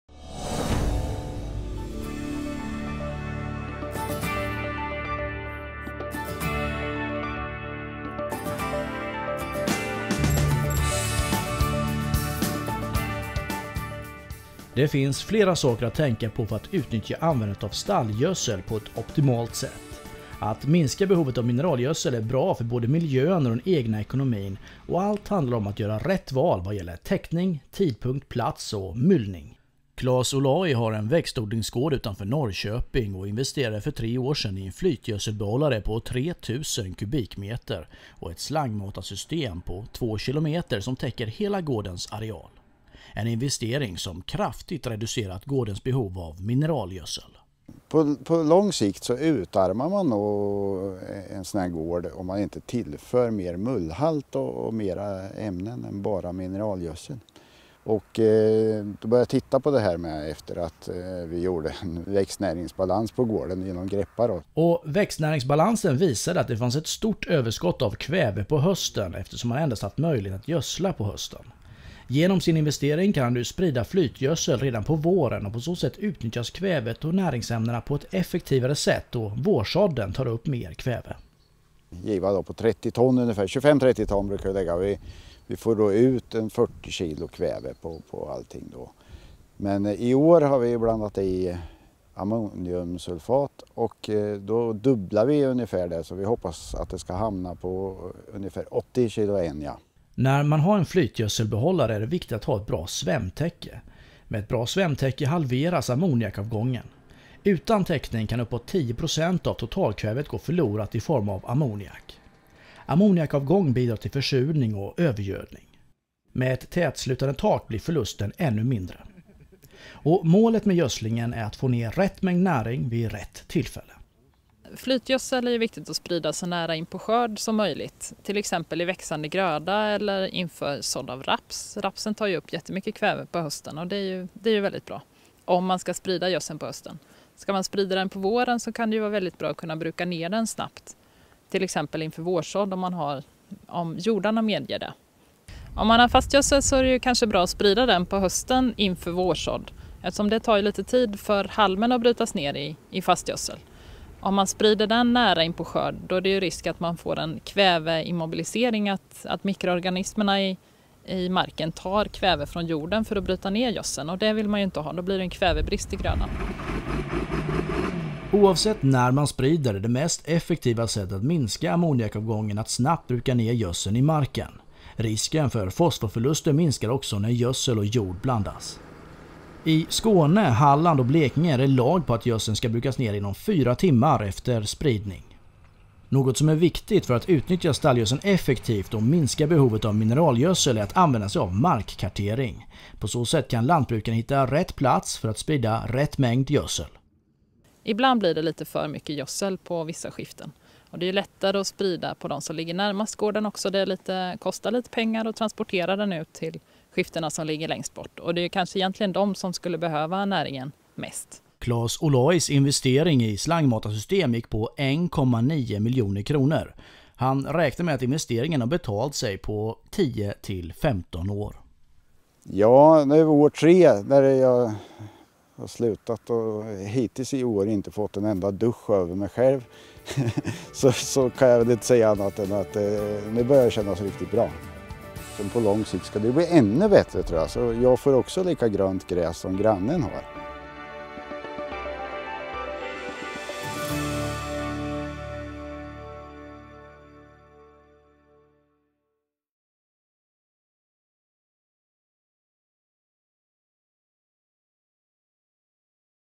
Stallgödsel
Få tips om hur du utnyttjar din stallgödsel på bästa sätt.